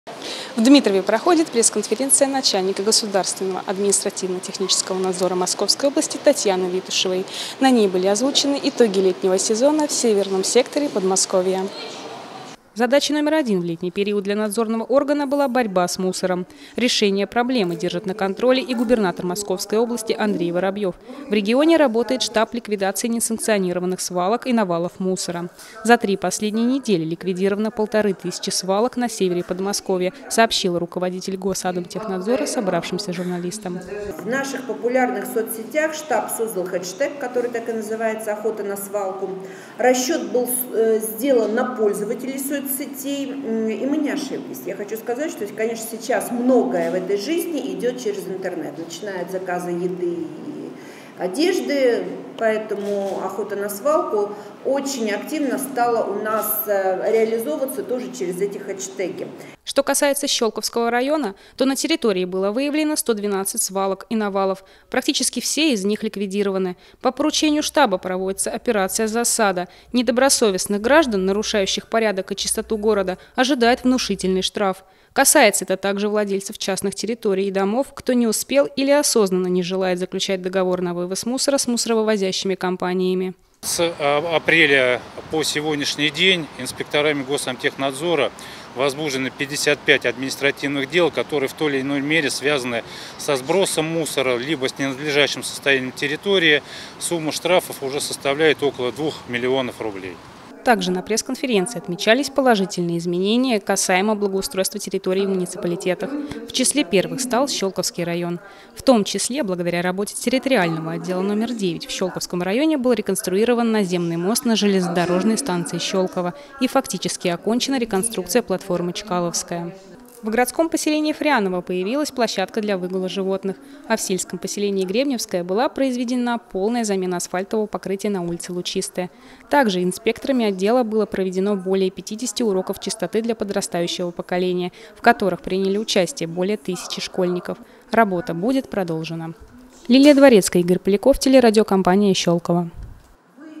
Итоги работы в летний период подвели сотрудники Госадмтенадзора Московской области
Итоги летнего сезона на севере Подмосковья подвели на  пресс-конференции главного государственного административно-технического инспектора Московской области Татьяны Витушевой. Расскажет наш корреспонднет.